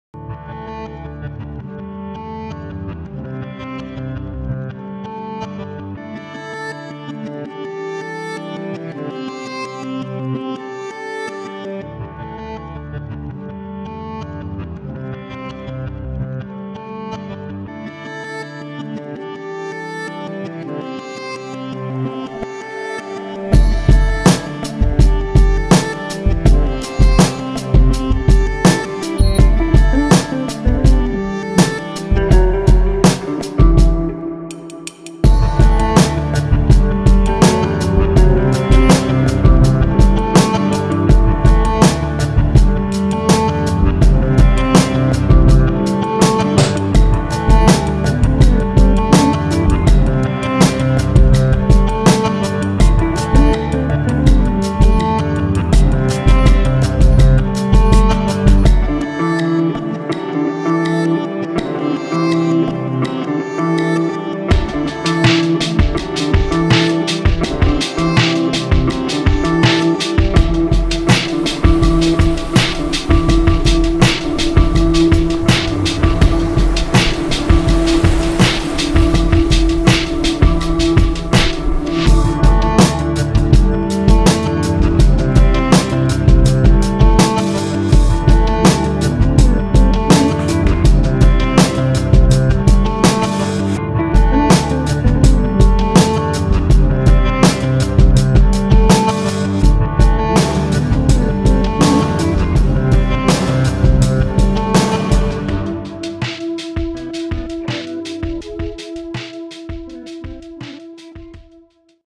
Hasegawa 1/48 Aichi D3A1 Type 99 Carrier Dive Bomber Val Model 11
Stellar build by Jeff Williams for Flory Models Japanese Group build!!